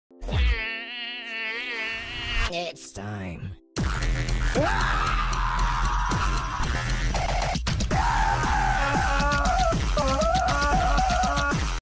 I zapped Windows millennium edition gamer 2000
Ha, ha! That’s what you get for Hateing Channel I Like Such as Cartoon Network  Disney xd Nickelodeon and Disney channel. and he Hate Shows I like such as Peppa Pig Family Guy The Ren & Stimpy Show and more